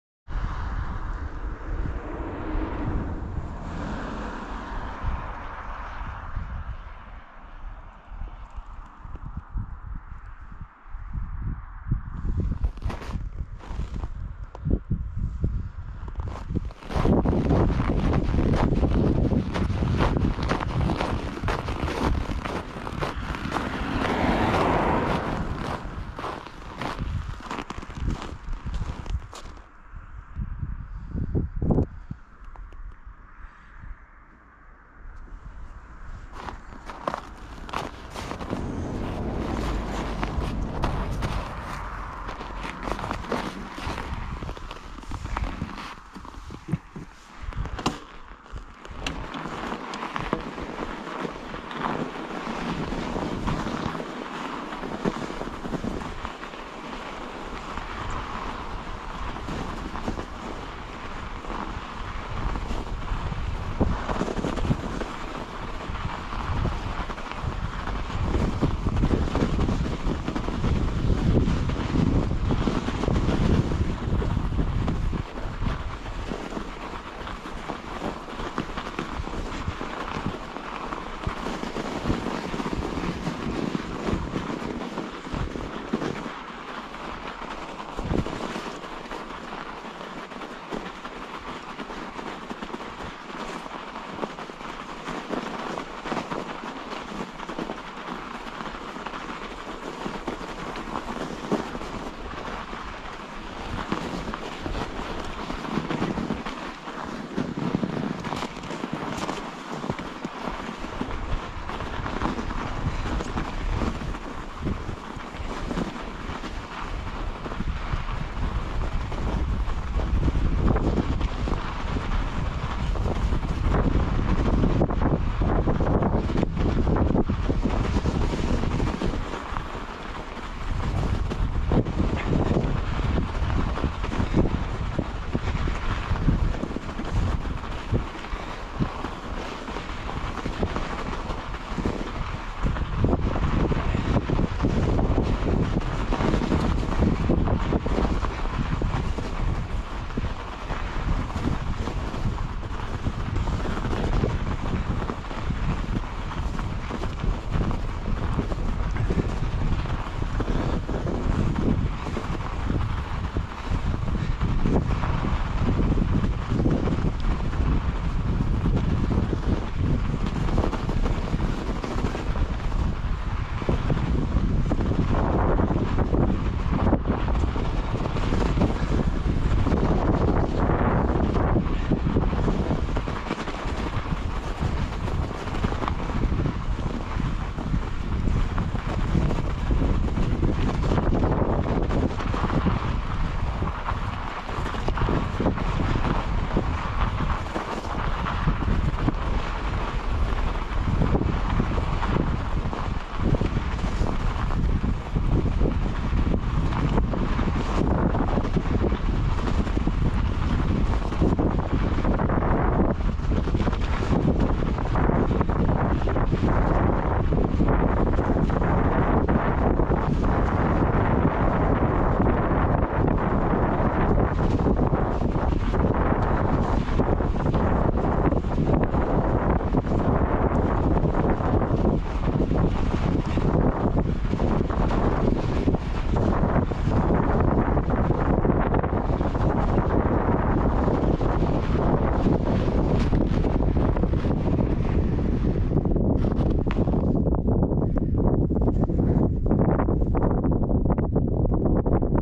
Esimene "Fatbike" jalgrattasõit Kiili vallas 07.12.2023
Video on filmitud kell 15:26.
Asukoht Sepamäe tee.